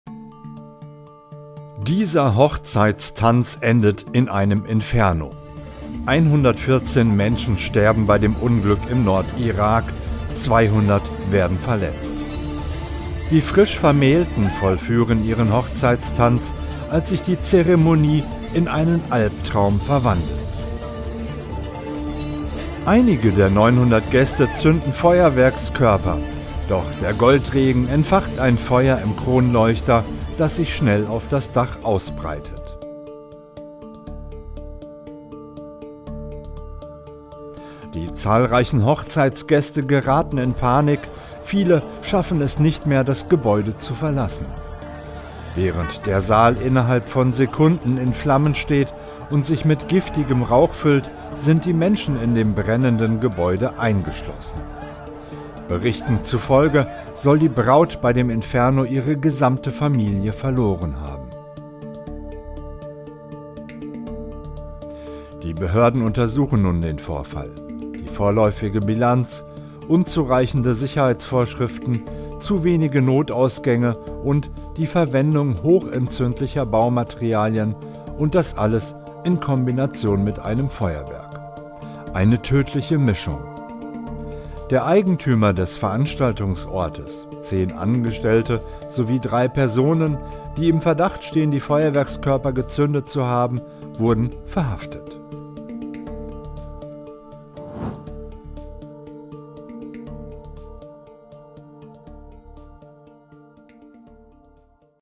Feuer-Tragödie: Hochzeits-Feier wird zur Todesfalle
Dieser Hochzeitstanz endet in einem Inferno. 114 Menschen sterben bei dem Unglück im Nord-Irak, 200 werden verletzt.